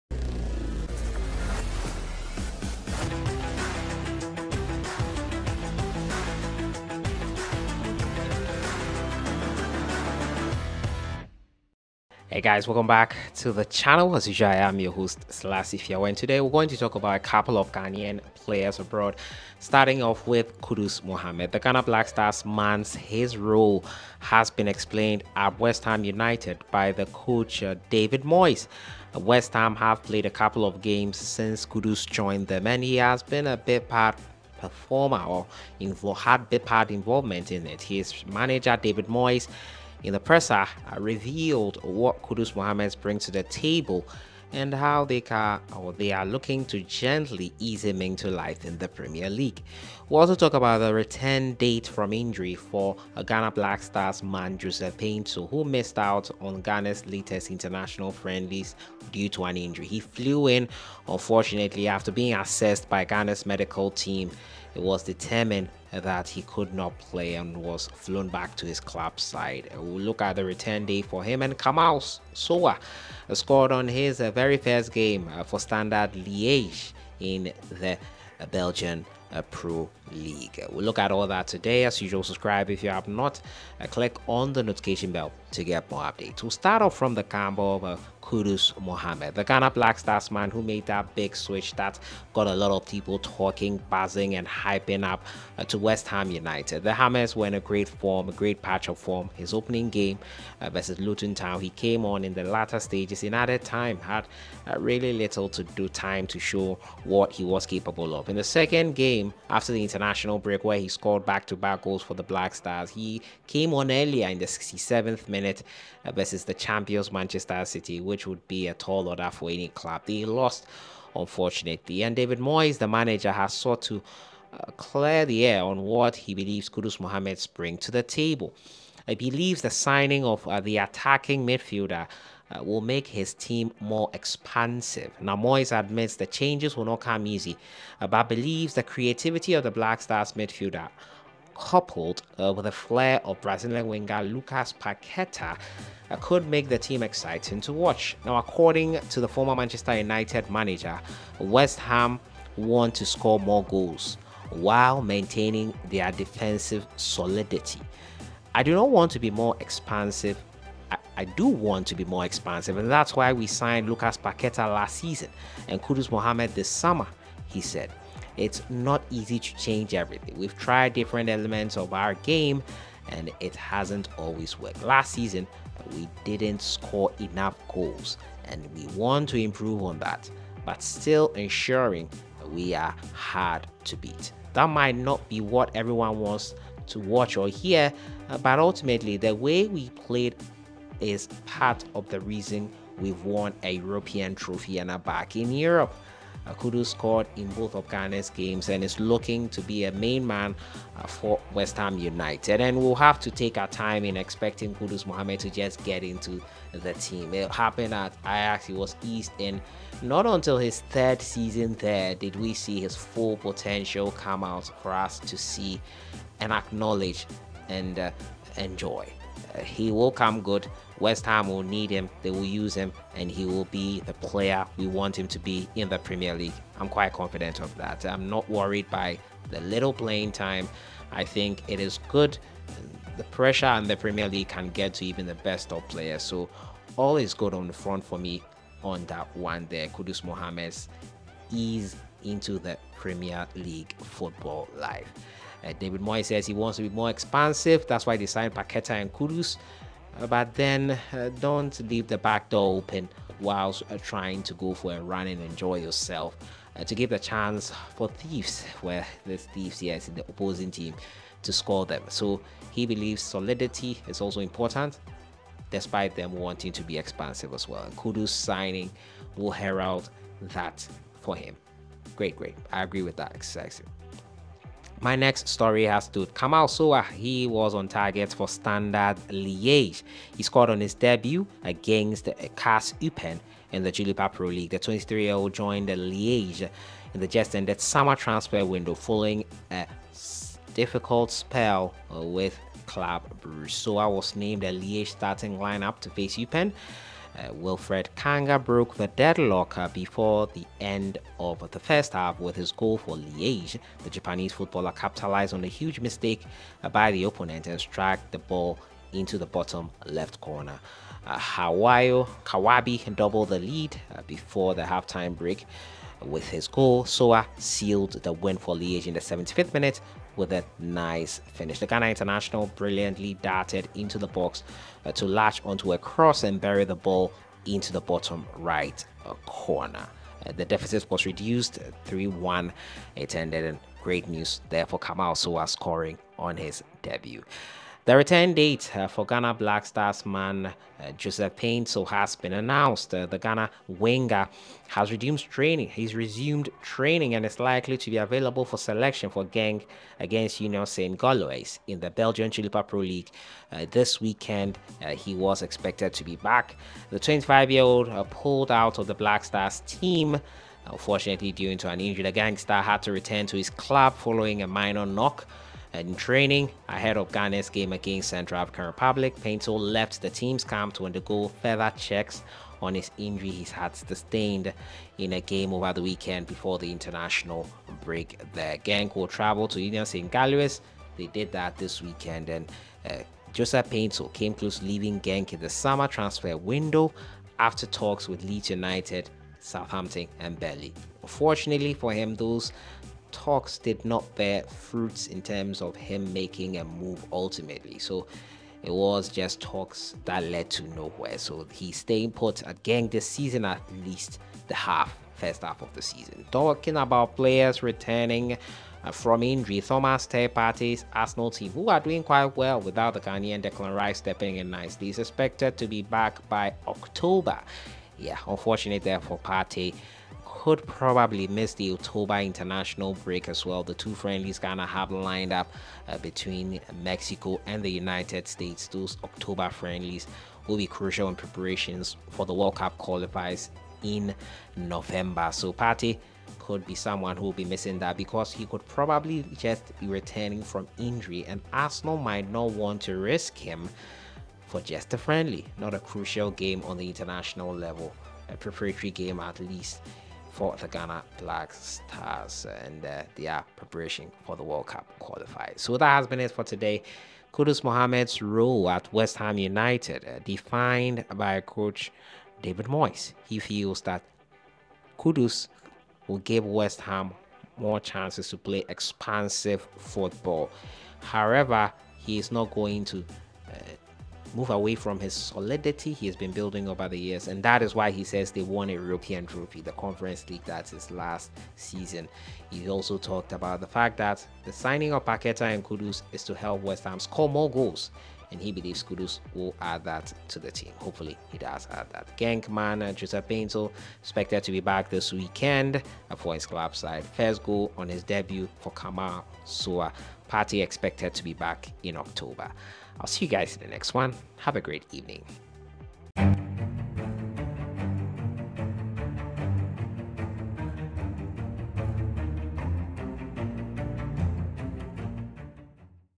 KUDUS MOHAMMED'S ROLE EXPLAINED BY COACH DAVID MOYES & KAMAL SOWAH ON TARGET IN DEBUT
WE LOOK AT THE ROLE OF KUDUS MOHAMMED FOR HIS NEW CLUB WEST HAM ACCORDING TO COACH DAVID MOYES & KAMAL SOWAH'S DEBUT GOAL FOR HIS NEW CLUB SIDE